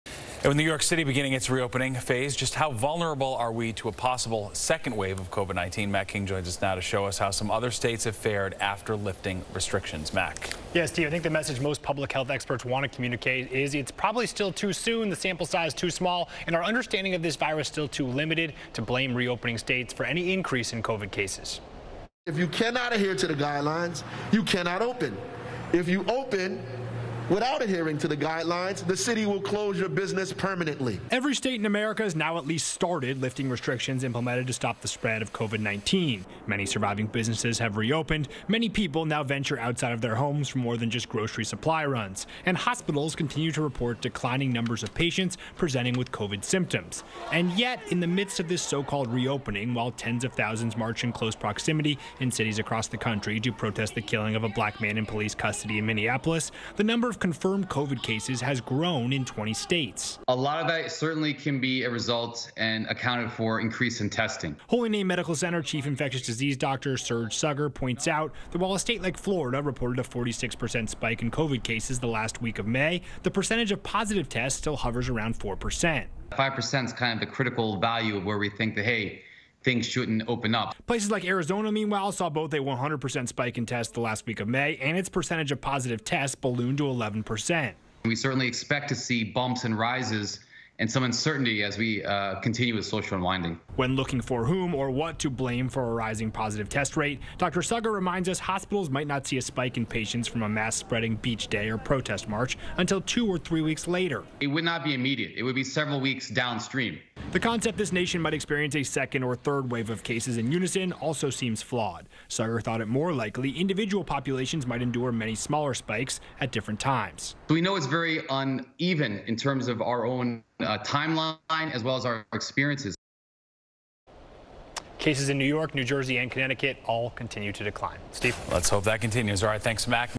Coronavirus Updates W/ Dr. Suraj Saggar on Fox 5 NY
As more public places reopen, how vulnerable are we to a possible second wave of coronavirus? Dr. Suraj Saggar, our chief of infectious disease, weighs in.